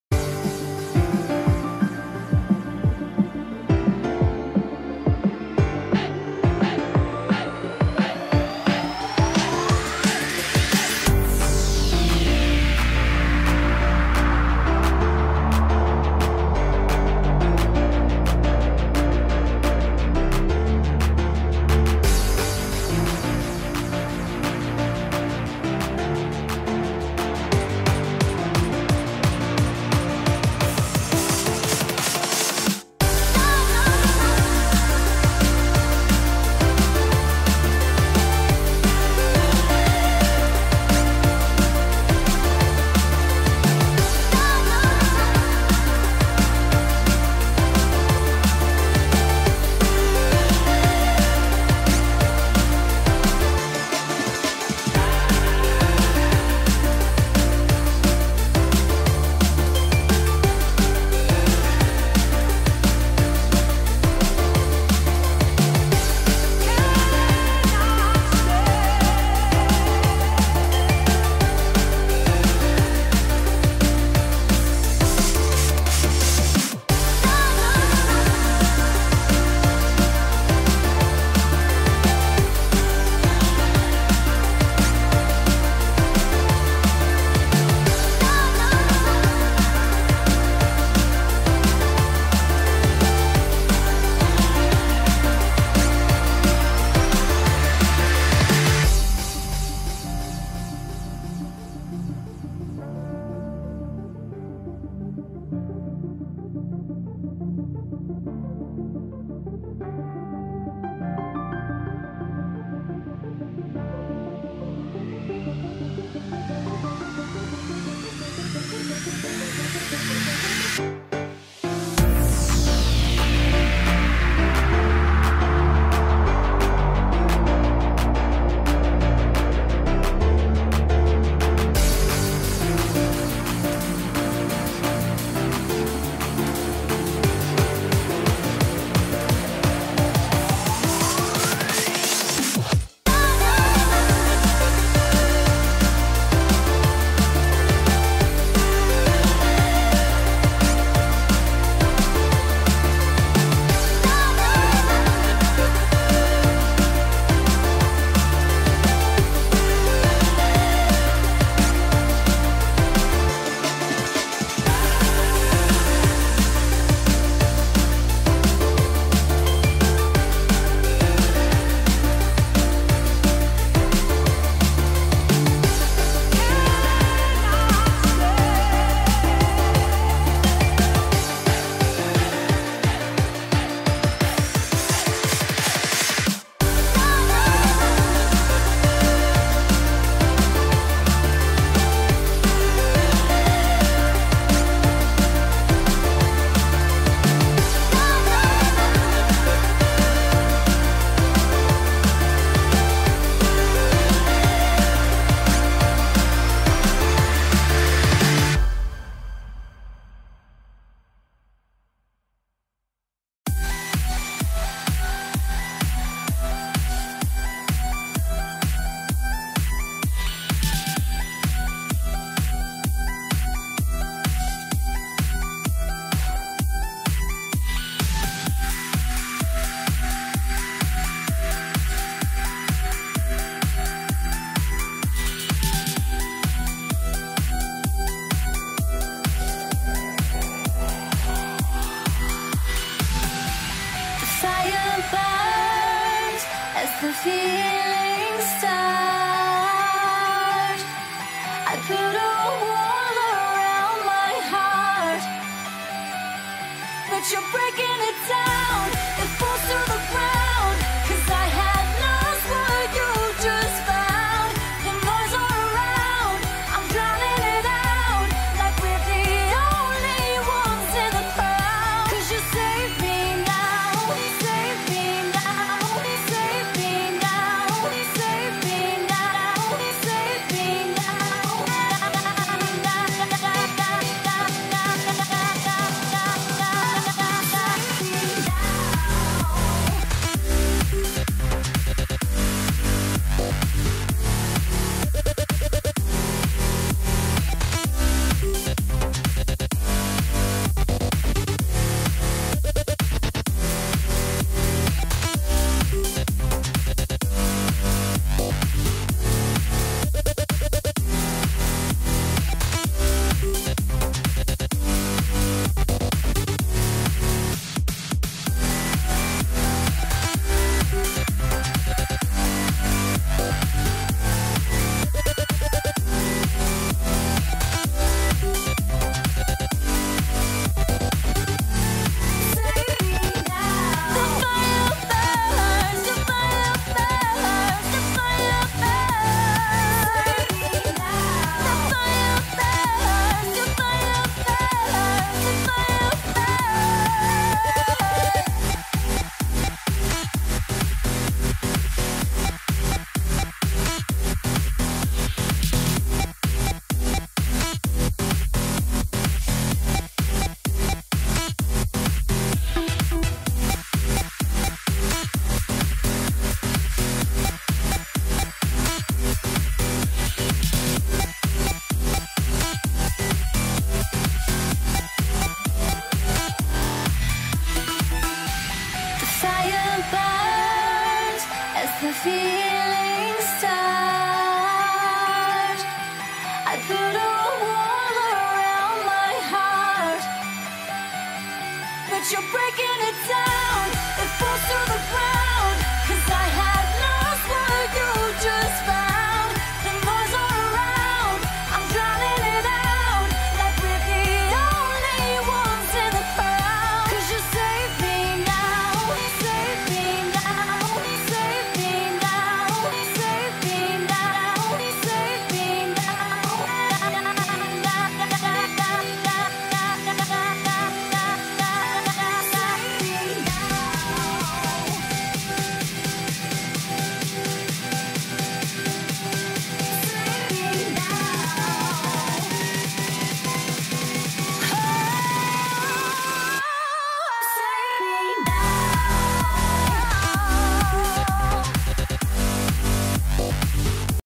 drawing on windows paint (Ahmed ata)
a hand sketch painting on windows paint by a simple mouse pointer